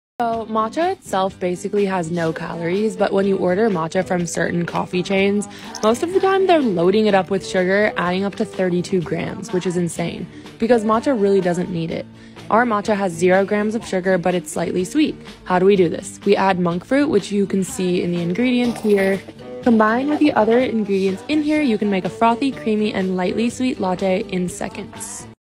All About Our Zero Sugar Matcha Powder
Matcha doesn’t need sugar, especially if it's good quality! Just a little bit of monk fruit does the trick for a perfectly balanced flavor 🍵 Sometimes we drink our matcha just with hot water. On other days, if we want it thicker/creamier, we add our Vanilla or Original Vegan Collagen Support Creamers!